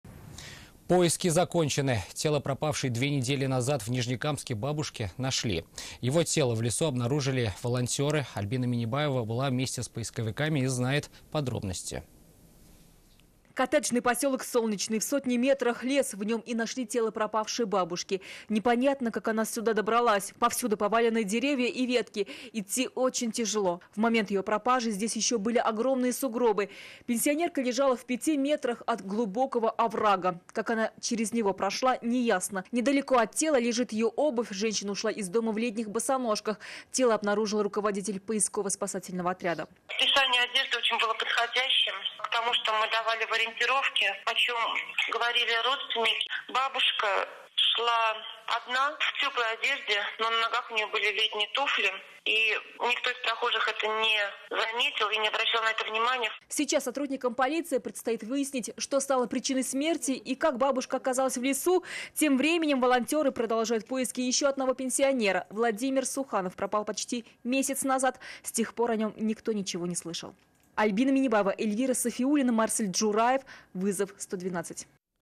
Тело пропавшей две недели назад в Нижнекамске бабушки нашли - ТНВ
Поиски закончены. Тело пропавшей две недели назад в Нижнекамске бабушки нашли. Его днём в лесу обнаружили волонтёры.

Таяну ноктасы. В эфире: пн, вт, ср, чт - 19:00

7 дней. В эфире: вс - 19:30, 21:30

Координаты:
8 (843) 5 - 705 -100